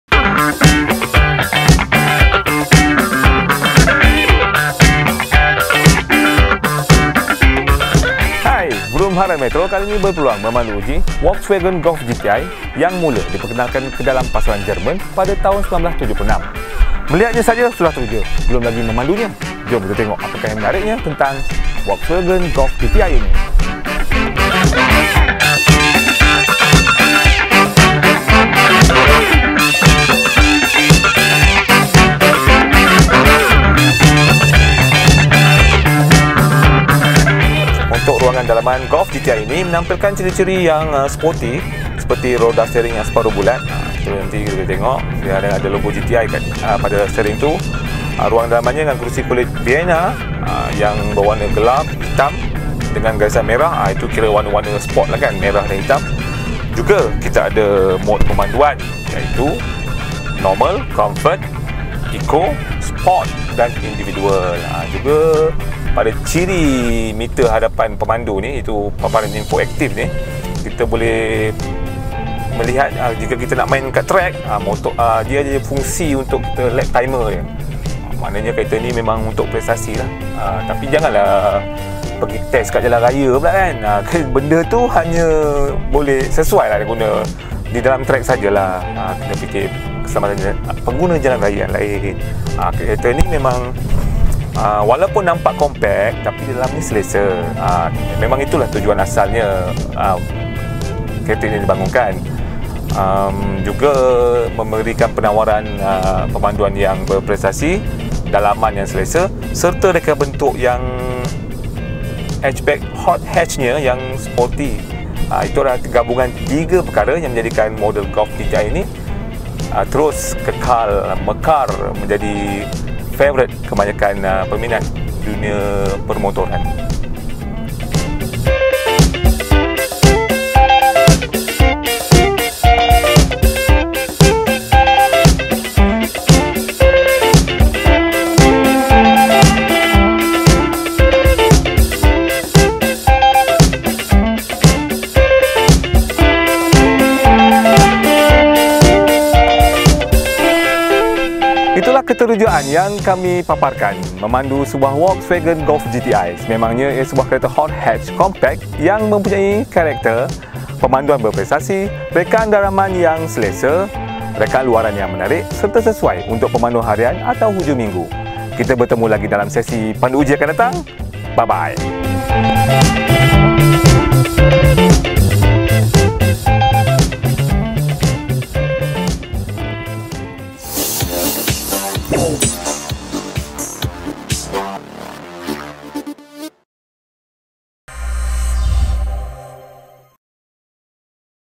Ikonik Jerman
VroomHM menguji ‘hot hatch’ Volswagen Golf GTI daripada generasi ke 7.5 (Mk 7.5).